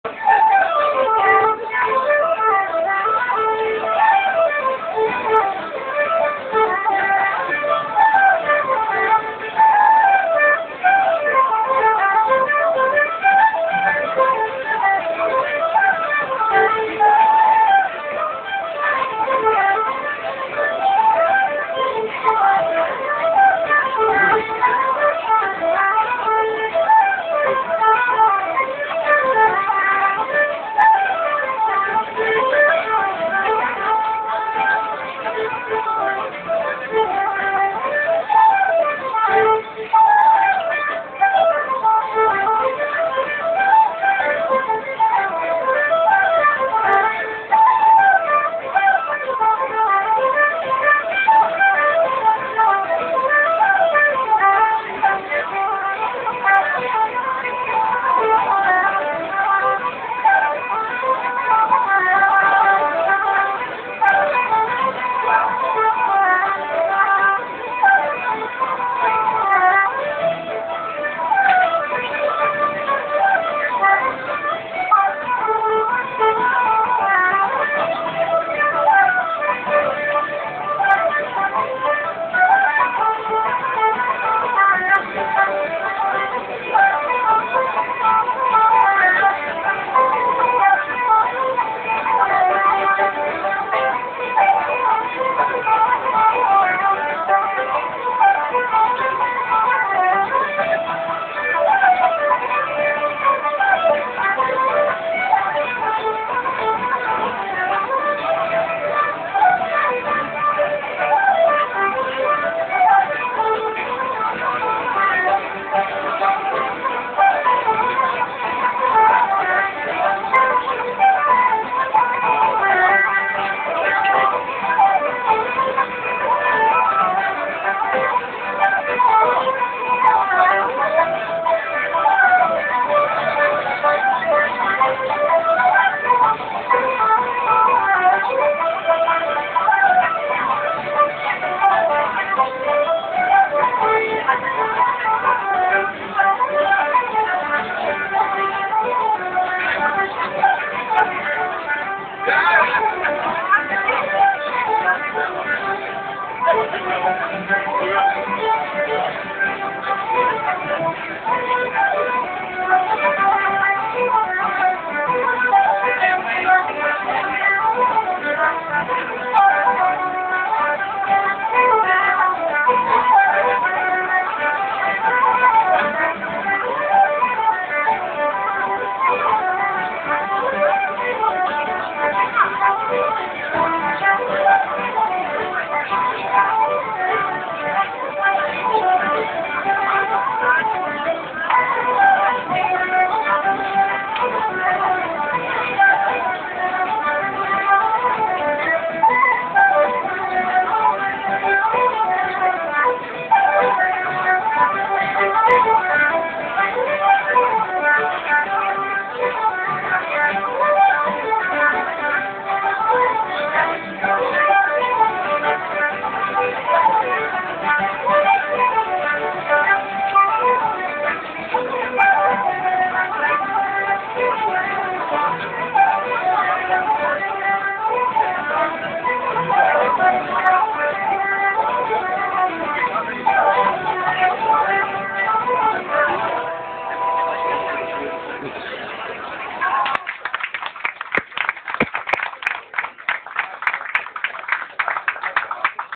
Emerald And Fans Maschseefest Hannover 14.8.2008
Irische Musik in der Temple Bar am Maschsee